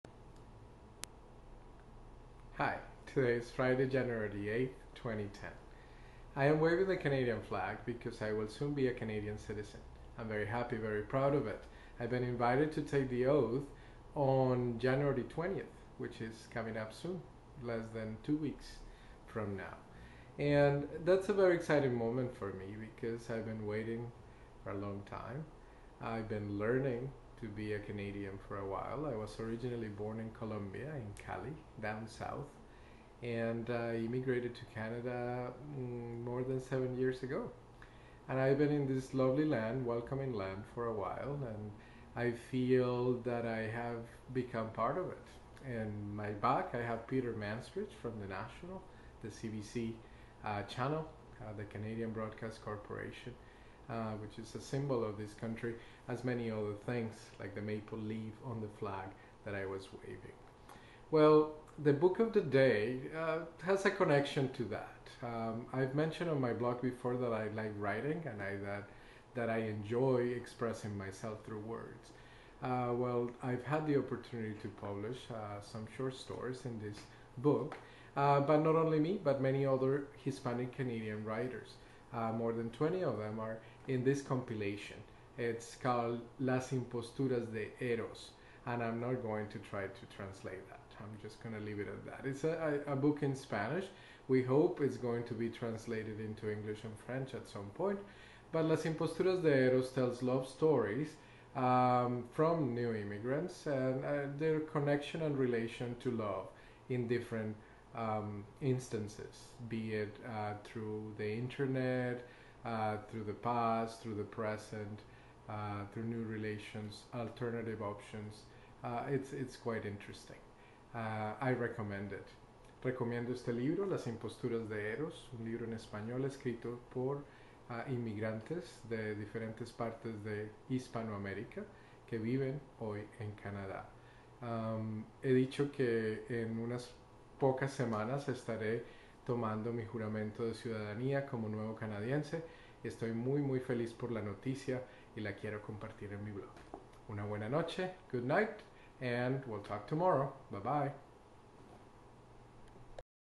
Canadian Citizensip - Las Imposturas de eros - Antonio's blog
Waving the Canadian flag and cheering for the Hispanic Canadian writiers. I will be taking the citizenship oath on January 20th, exciting!